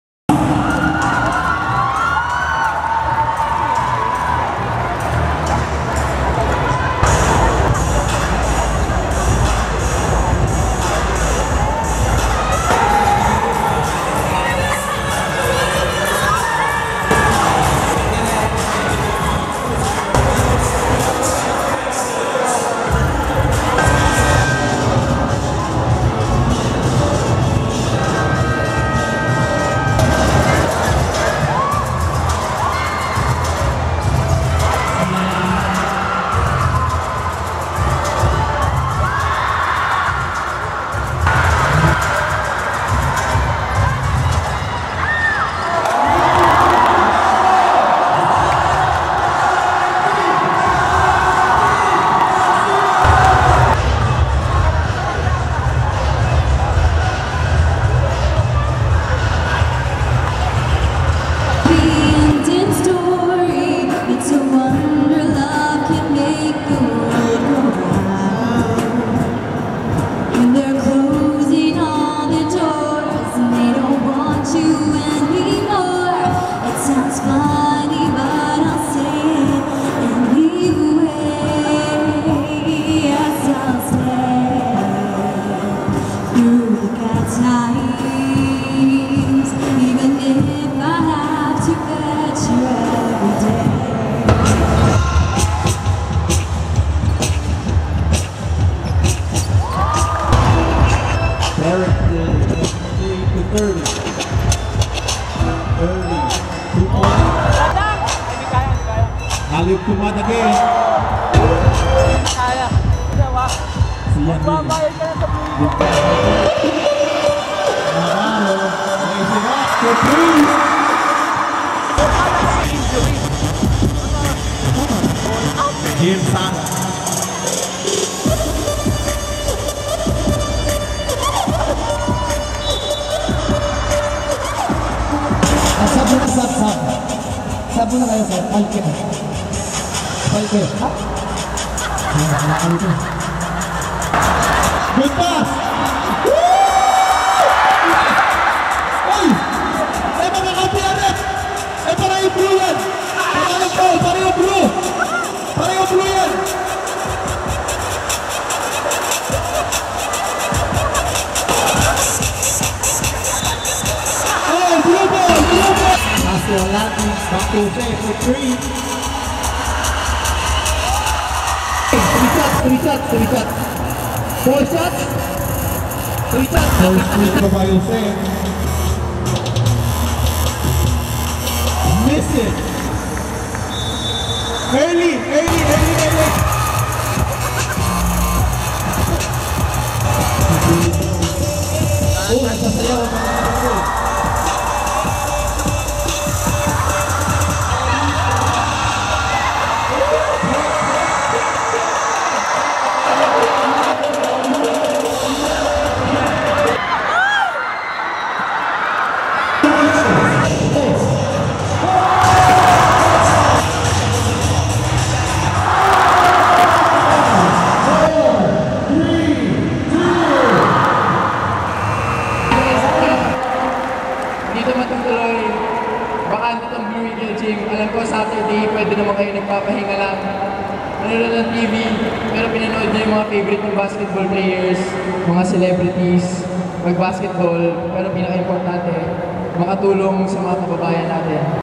Fastbreak 2: Charity match for 'Yolanda' victims
Matt Ganuelas provided the early highlights while comedian Jose Manalo provided the non-stop comic relief in a fun charity game which fittingly ended in a draw. Video by INQUIRER.net's Celest Flores